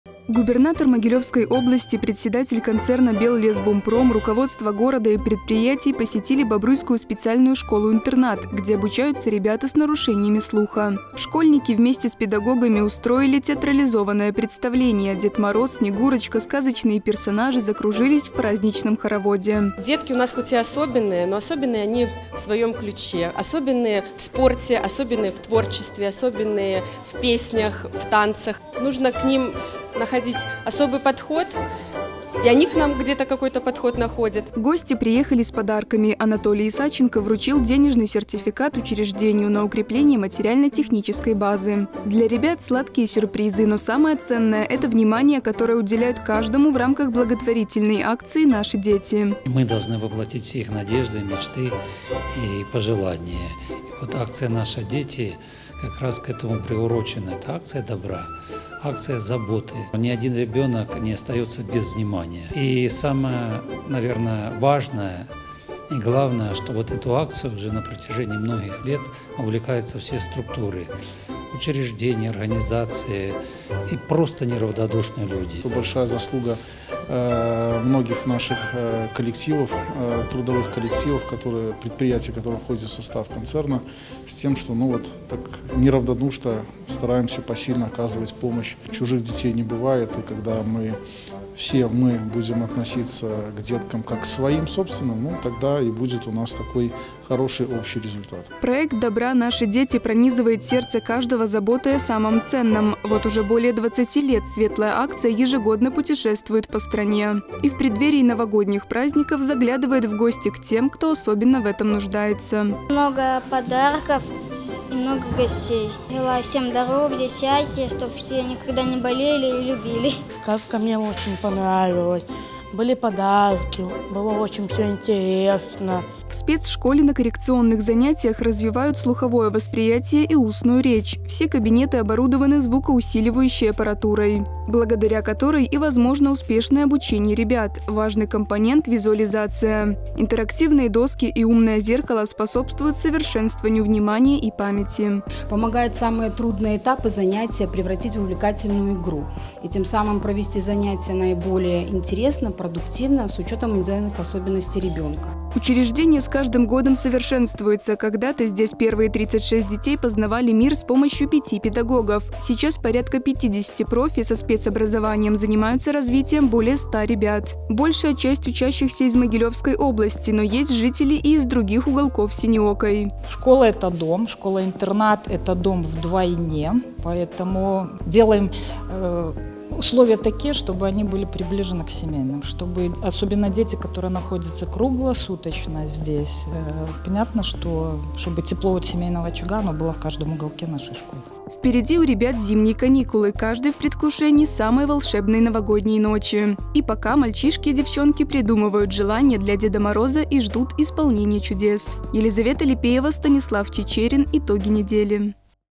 Время сказки
Акция «Наши дети» заглянула в Бобруйскую специальную школу-интернат.